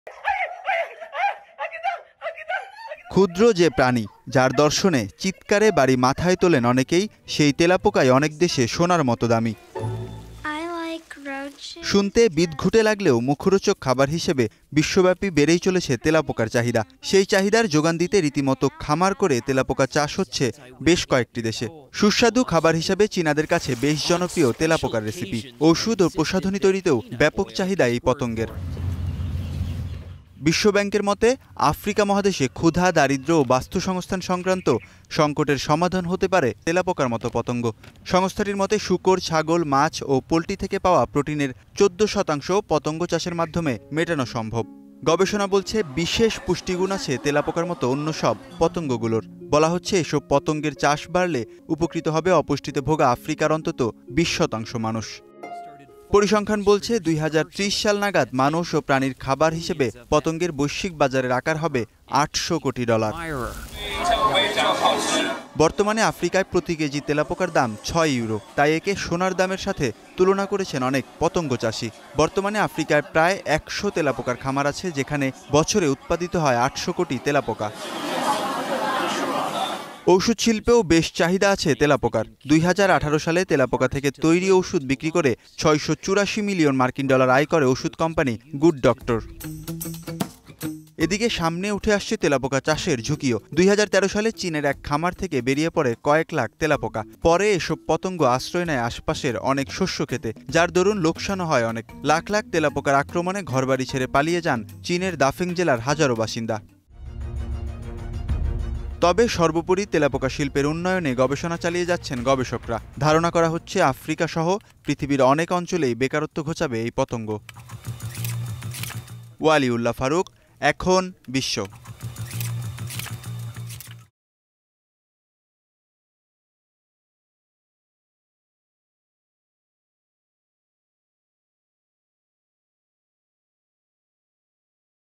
তেলাপোকা চাষে কমবে বেকারত্ব! | Cockroach | Ekhon TV
বিশ্বজুড়ে খাদ্য তালিকায় চাহিদা বাড়ছে তেলাপোকার। ওষুধ ও প্রসাধনী তৈরিতেও ব্যাপক চাহিদা এই পতঙ্গের। বলা হচ্ছে, ২০৩০ সাল নাগাদ খাবার হিসেবে তেলাপোকার মতো পতঙ্গের বৈশ্বিক বাজার হবে ৮০০ কোটি ডলার।

Our Office Address:
EKHON TV
City Park Lane
19 Hatkhola Road, Wari
Dhaka-1209
Bangladesh